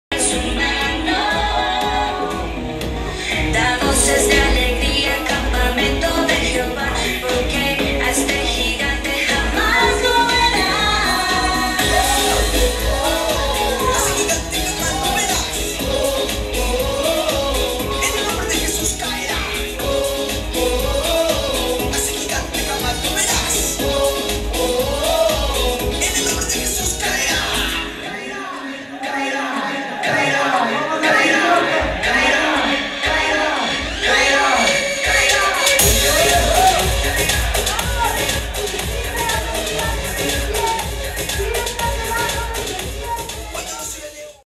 Aviva León Todos somos barra!
Reuniones de Avivamiento en Cinépolis Centro Max 9:30 am, León Guanajuato. Entrada Libre. Los Esperamos! Llénate de entusiasmo!